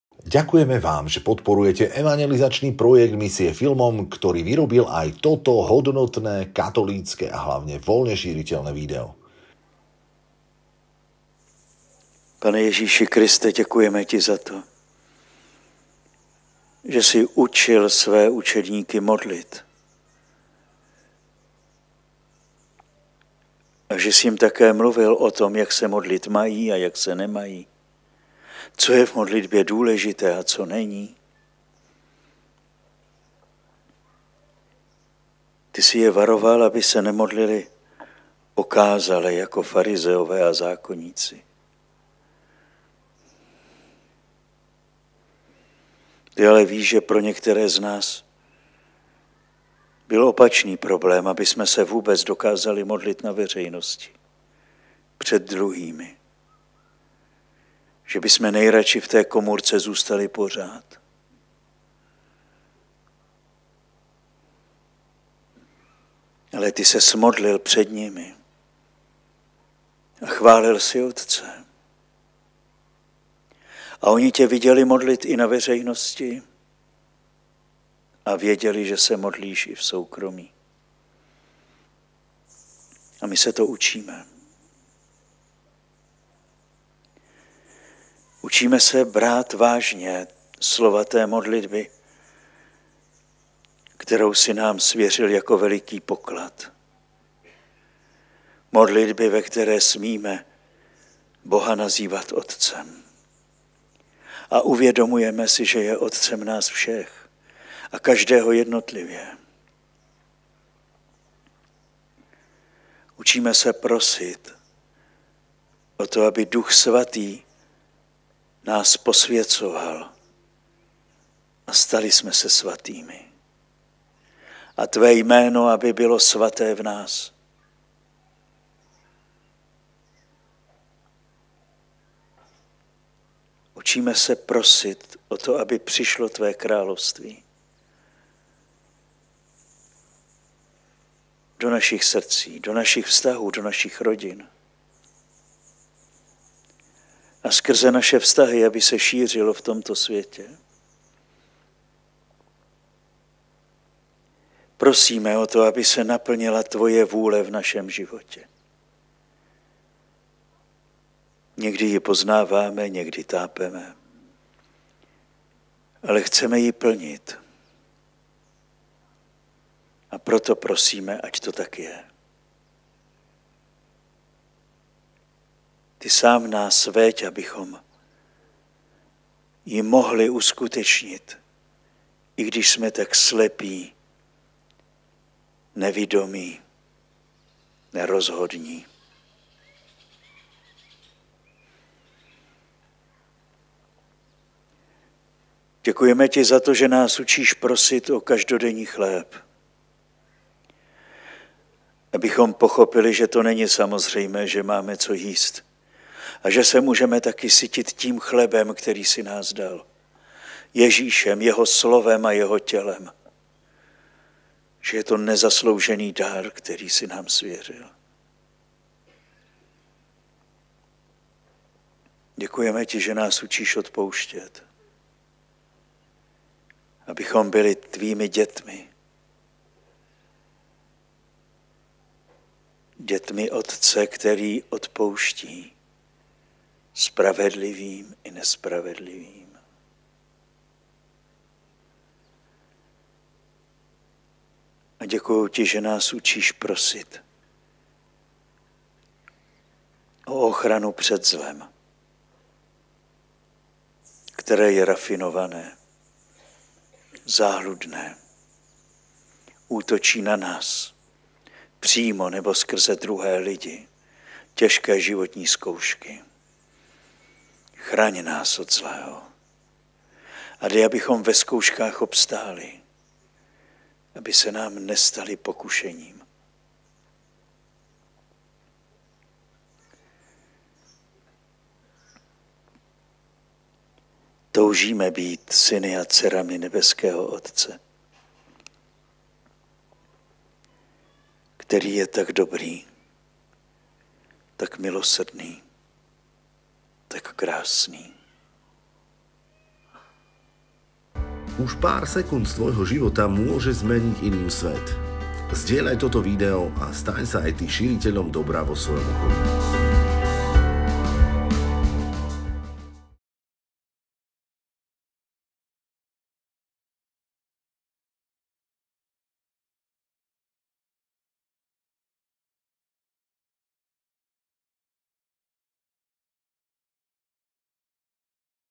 o. Kodet - Modlitba pred sviatosťou #3
Sledujete sériu krátkych modlitieb o. Vojtěcha Kodeta pred sviatosťou oltárnou.

Ako sa máme modliť OTČE NÁŠ❓

Nauč nás, Pane, brať vážne slová modlitby, ktorú si nám zveril ako veľký poklad;

🙏modlitby, v ktorej môžeme nazývať Boha našim Otcom;
❤️toho, ktorý je Otcom nás všetkých a zároveň každého jednotlivo.

(porov. o. Kodet)